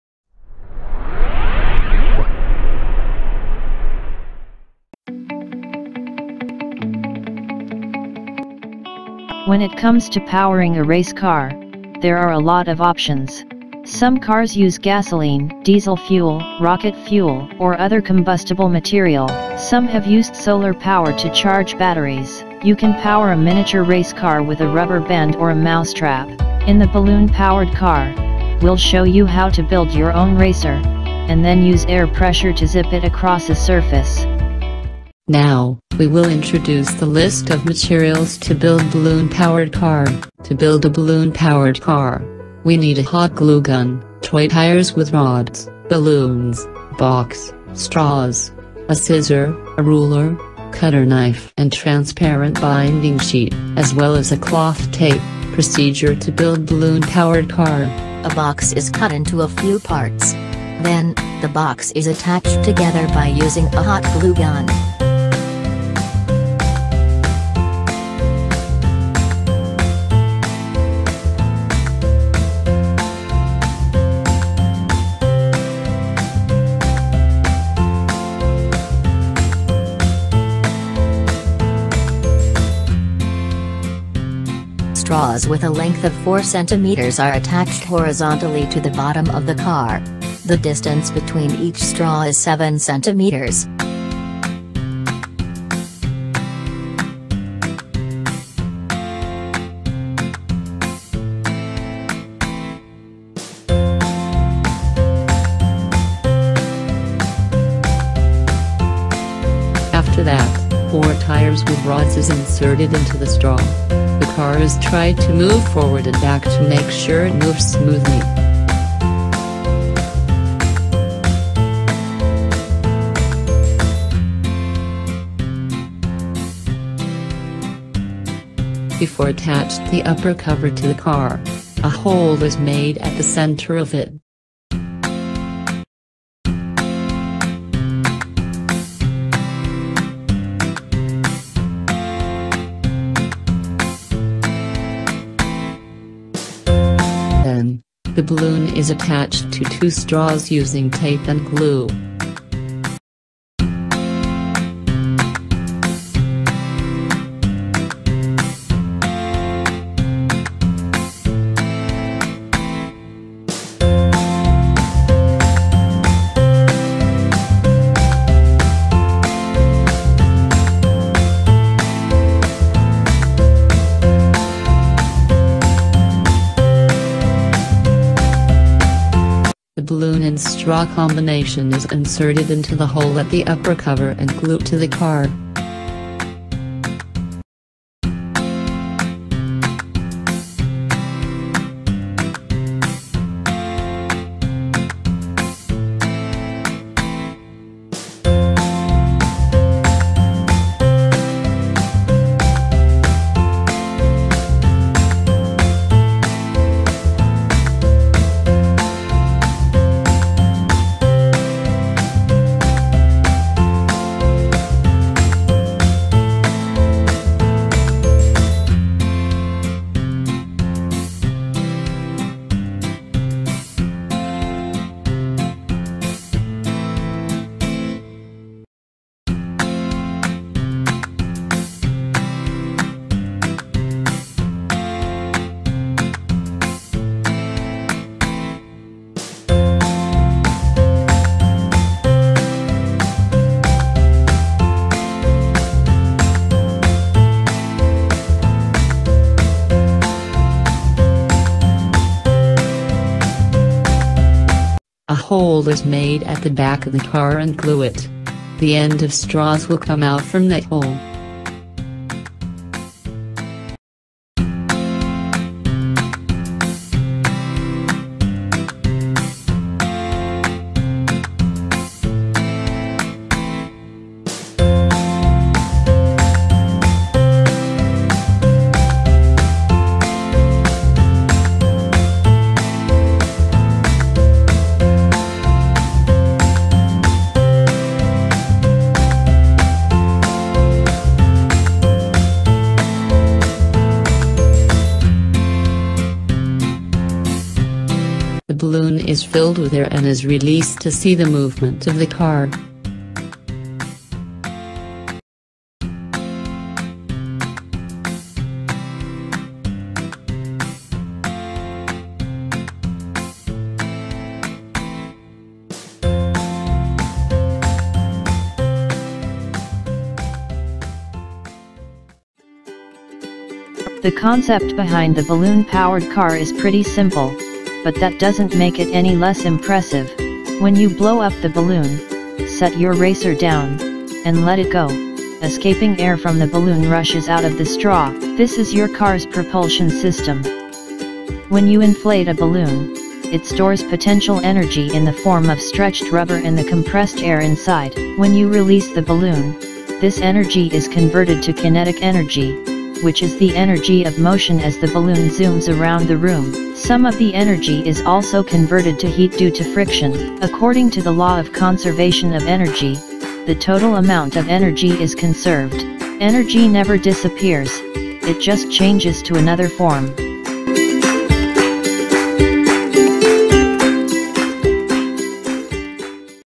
Group Project: Balloon Powered Car
SFT4031: ENERGY PHYSICS LAB
Powered Car System Project
Lecturer: Dr. Nadhrah Md. Yatim